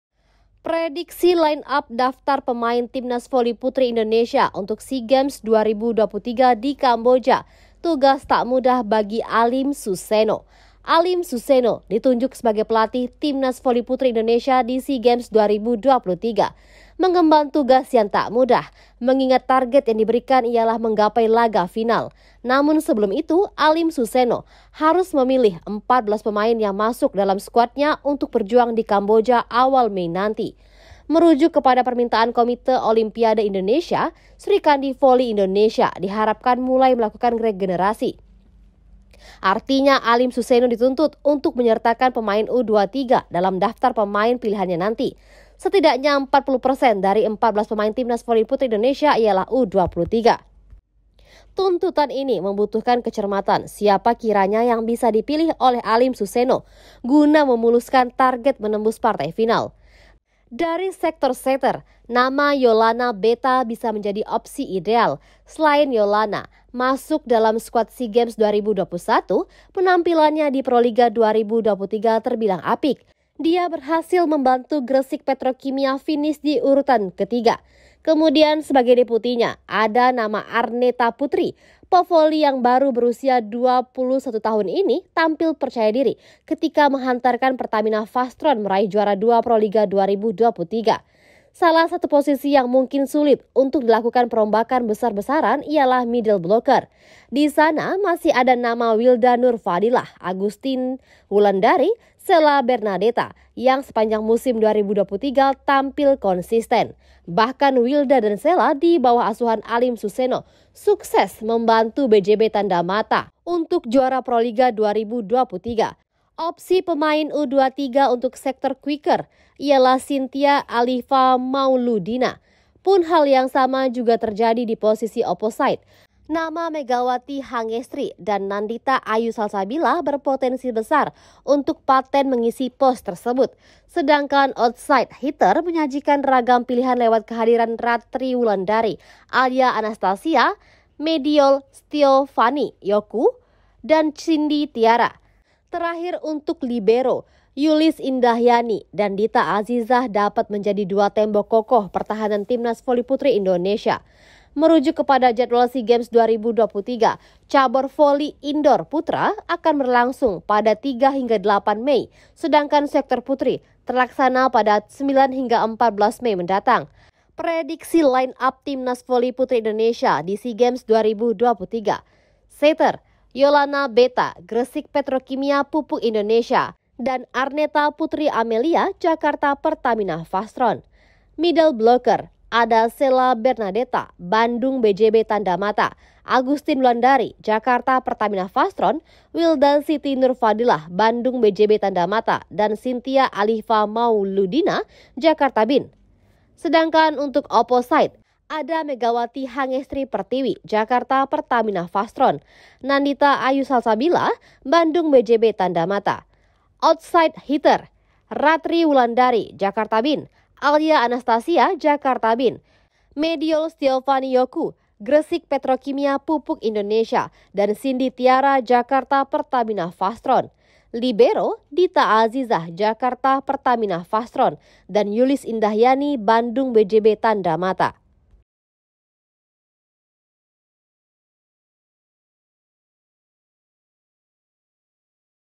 🔴 Prediksi Line-up Timnas Voli Putri Indonesia di SEA Games 2023: Gerbong Juara Proliga 2023 Dominan
Prediksi line-up daftar pemain Timnas voli putri Indonesia untuk SEA Games 2023 di Kamboja, tugas tak mudah bagi Alim Suseno.

Alim Suseno ditunjuk sebagai pelatih Timnas voli putri Indonesia di SEA Games 2023, mengemban tugas yang tak mudah, mengingat target yang diberikan ialah menggapai laga final.

Namun sebelum itu, Alim Suseno harus memilih 14 pemain yang masuk dalam skuadnya untuk berjuang di Kamboja awal Mei nanti.

Merujuk kepada permintaan Komite Olimpiade Indonesia (KOI), srikandi voli Indonesia diharapkan mulai melakukan regenerasi.

Artinya, Alim Suseno dituntut untuk menyertakan pemain U23 dalam daftar pemain pilihannya nanti.  Setidaknya 40 persen dari 14 pemain Timnas voli putri Indonesia ialah U23.

Tuntutan ini membutuhkan kecermatan siapa kiranya yang bisa dipilih oleh Alim Suseno guna memuluskan target menembus partai final.

Dari sektor setter, nama Yollana Betha bisa menjadi opsi ideal. Selain Yolana masuk dalam skuad SEA Games 2021, penampilannya di Proliga 2023 terbilang apik.

Dia berhasil memantu Gresik Petrokimia finis di urutan ketiga.

Kemudian sebagai deputinya, ada nama Arnetta Putri. Pevoli yang baru berusia 21 tahun ini tampil percaya diri ketika menghantarkan Pertamina Fastron meraih juara dua Proliga 2023.

Salah satu posisi yang mungkin sulit untuk dilakukan perombakan besar-besaran ialah middle blocker.

Di sana masih ada nama Wilda Nurfadhilah, Agustin Wulandhari, Shella Bernadetha yang sepanjang musim 2023 tampil konsisten.

Bahkan Wilda dan Shella di bawah asuhan Alim Suseno sukses membantu BJB Tandamaya juara Proliga 2023. Opsi pemain U23 untuk sektor quicker ialah Shintia Alliva Mauludina.

Pun hal yang sama juga terjadi di posisi opposite. Nama Megawati Hangestri dan Nandita Ayu Salsabila berpotensi besar untuk paten mengisi pos tersebut.

Sedangkan outside hitter menyajikan ragam pilihan lewat kehadiran Ratri Wulandari, Alya Anastasia, Mediol Stiovanny Yoku, dan Cindy Tiara.

Terakhir untuk libero, Yulis Indahyani dan Dita Azizah dapat menjadi dua tembok kokoh pertahanan Timnas voli putri Indonesia.

Merujuk kepada jadwal SEA Games 2023, cabor voli indoor putra akan berlangsung pada 3-8 Mei. Sedangkan sektor putri terlaksana pada 9-14 Mei mendatang.

Prediksi Line-up Timnas Voli Putri Indonesia di SEA Games 2023

Setter

- Yolana Betha (Gresik Petrokimia Pupuk Indonesia)
- Arnetta Putri Amelia (Jakarta Pertamina Fastron)

Middle Blocker

- Shella Bernadetha (Bandung BJB Tandamata)
- Agustin Wulandhari (Jakarta Pertamina Fastron)
- Wilda Siti Nurfadhillah (Bandung BJB Tandamata)
- Shintia Alliva Mauludina (Jakarta BIN)

Opposite

- Megawati Hangestri Pertiwi (Jakarta Pertamina Fastron)
- Nandita Ayu Salsabila (Bandung BJB Tandamata)

Outside Hitter

- Ratri Wulandari (Jakarta BIN)
- Alya Anastasia (Jakarta BIN)
- Mediol Stiovanny Yoku (Gresik Petrokimia Pupuk Indonesia)
- Cindy Tiara (Jakarta Pertamina Fastron)

Libero

- Dita Azizah (Jakarta Jakarta Pertamina Fastron)
- Yulis Indahyani (Bandung BJB Tandamata)